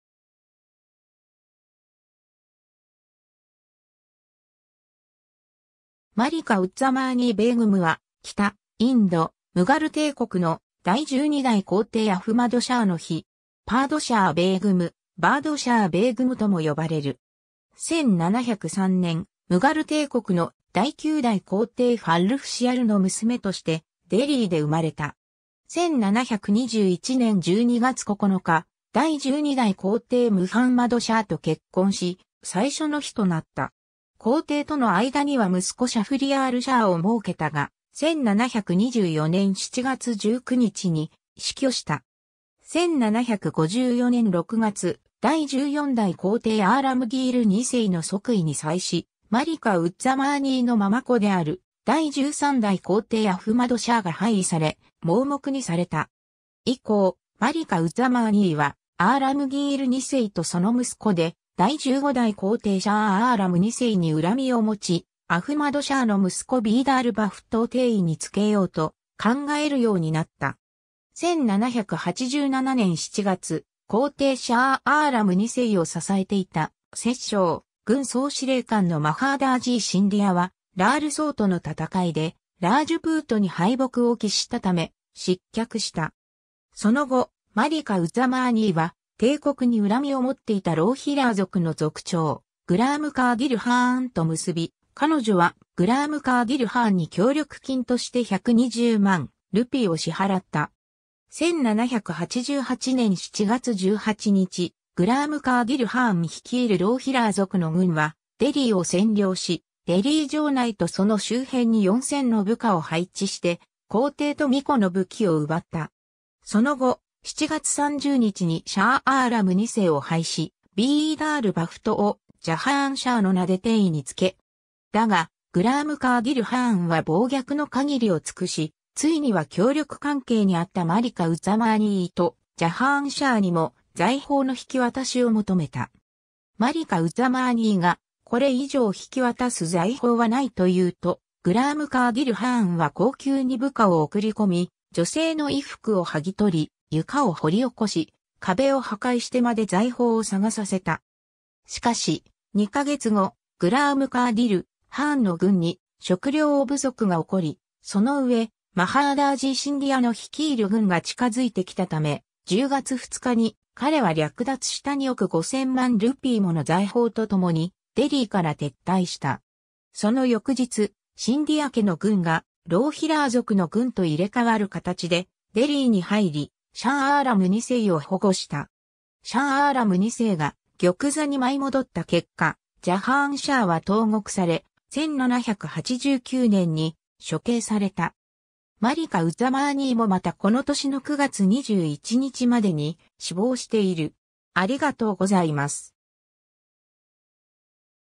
マリカ・ウッザマーニー・ベーグム
マリカ・ウッザマーニー・ベーグム（Malika uz-Zamani Begum, 1703年 - 1789年9月21日以前）は、北インド、ムガル帝国の第12代皇帝アフマド・シャーの妃。
パードシャー・ベーグム（Padshah Begum）、バードシャー・ベーグム（Badshah Begum）とも呼ばれる。
 1703年、ムガル帝国の第9代皇帝ファッルフシヤルの娘として、デリーで生まれた。
 1721年12月9日、第12代皇帝ムハンマド・シャーと結婚し、最初の妃となった。
皇帝との間には息子シャフリヤール・シャーをもうけたが、1724年7月19日に死去した。
 1754年6月、第14代皇帝アーラムギール2世の即位に際し、マリカ・ウッザマーニーの継子である第13代皇帝アフマド・シャーが廃位され、盲目にされた。
以降、マリカ・ウッザマーニーはアーラムギール2世とその息子で第15代皇帝シャー・アーラム2世に恨みを持ち、アフマド・シャーの息子ビーダール・バフトを帝位につけようと考えるようになった。
 1787年7月、皇帝シャー・アーラム2世を支えていた摂政・軍総司令官のマハーダージー・シンディアはラールソートの戦いでラージプートに敗北を喫したため、失脚した。
 その後、マリカ・ウッザマーニーは帝国に恨みを持っていたローヒラー族の族長グラーム・カーディル・ハーンと結び、彼女はグラーム・カーディル・ハーンに協力金として120万ルピーを支払った。
 1788年7月18日、グラーム・カーディル・ハーン率いるローヒラー族の軍はデリーを占領し、デリー城内とその周辺に4，000の部下を配置して、皇帝と皇子の武器を奪った。
その後、7月30日にシャー・アーラム2世を廃し、ビーダール・バフトを「ジャハーン・シャー」の名で帝位につけ、。
 だが、グラーム・カーディル・ハーンは暴虐の限りを尽くし、ついには協力関係にあったマリカ・ウッザマーニーとジャハーン・シャーにも財宝の引き渡しを求めた。
マリカ・ウッザマーニーが「これ以上引き渡す財宝はない」と言うと、グラーム・カーディル・ハーンは後宮に部下を送り込み、女性の衣服を剥ぎ取り、床を掘り起こし、壁を破壊してまで財宝を探させた。
 しかし、2ヶ月後、グラーム・カーディル・ハーンの軍に食糧不足が起こり、そのうえマハーダージー・シンディアの率いる軍が近づいてきたため、10月2日に彼は略奪した2億5000万ルピーもの財宝とともにデリーから撤退した。
その翌日、シンディア家の軍がローヒラー族の軍と入れ替わる形でデリーに入り、シャー・アーラム2世を保護した。
 シャー・アーラム2世が玉座に舞い戻った結果、ジャハーン・シャーは投獄され、1789年に処刑された。
マリカ・ウッザマーニーもまたこの年の9月21日までに死亡している。